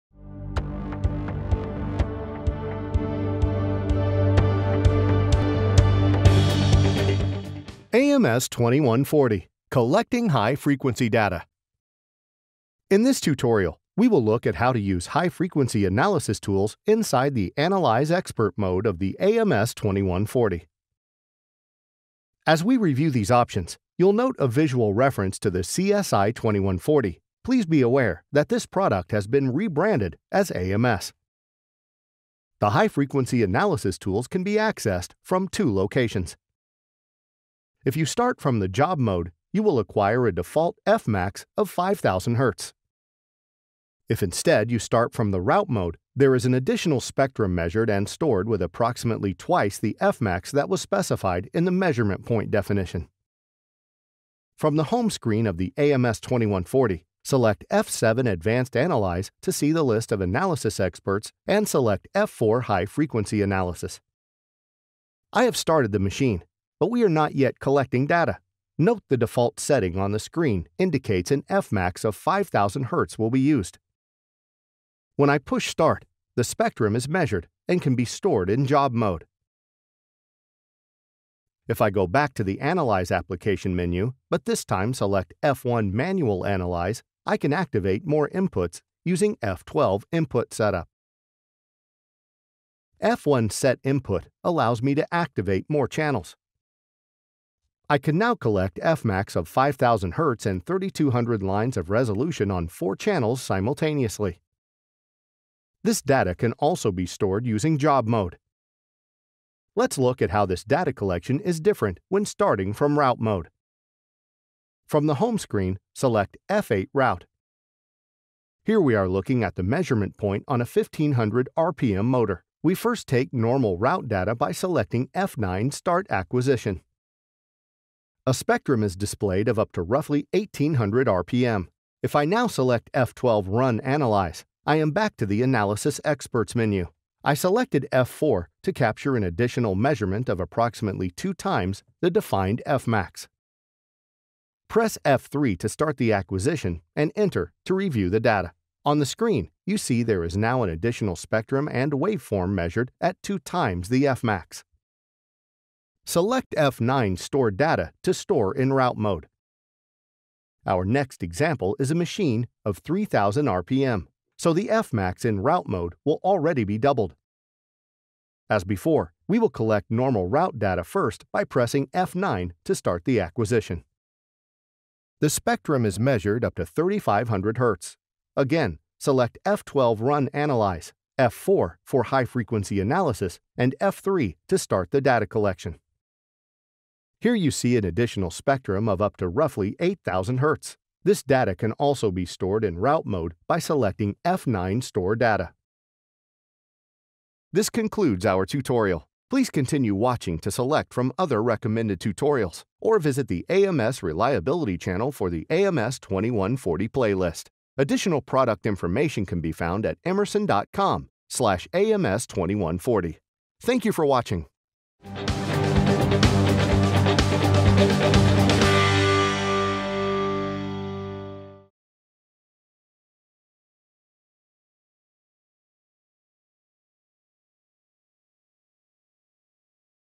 Tutorial: Collecting High Frequency Data Using the AMS 2140
This video tutorial details how to collect high frequency data for analysis in the AMS 2140 Machinery Health Analyzer, Analyzer/Expert mode. (Note: the AMS 2140 was previously branded as the CSI 2140. The rebranding has not impacted functionality or the accuracy of this tutorial.)